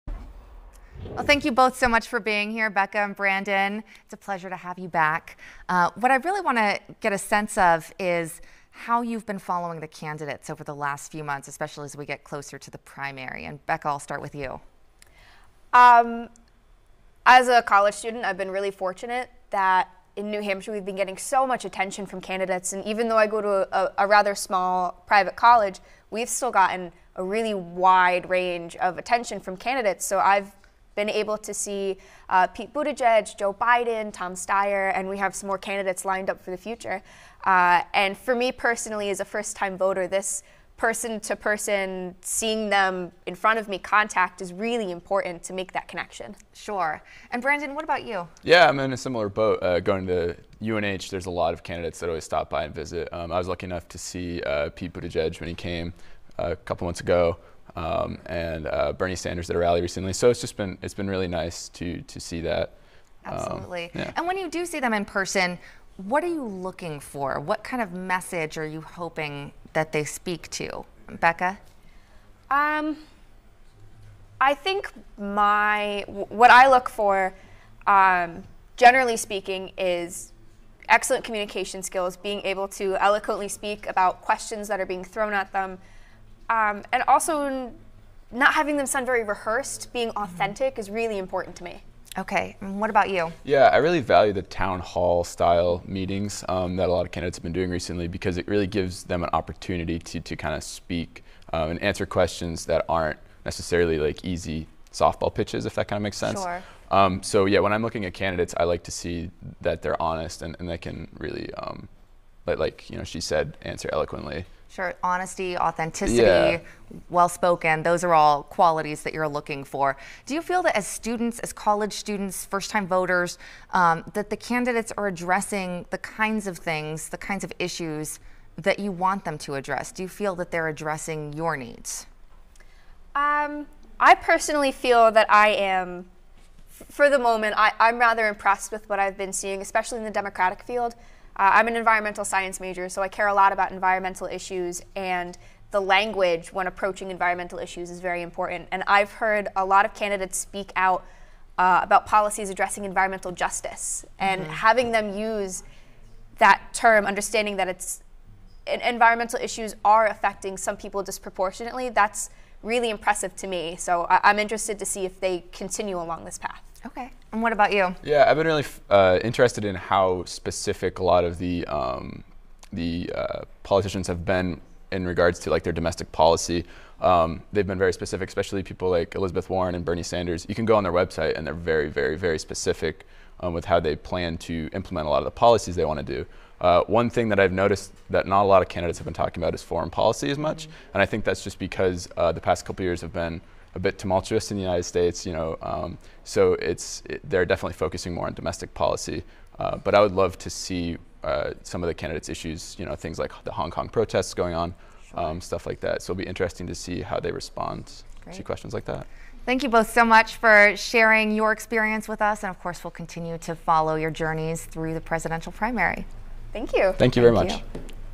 College students say they're following presidential race closely
New Hampshire college students share their thoughts on the presidential race so far.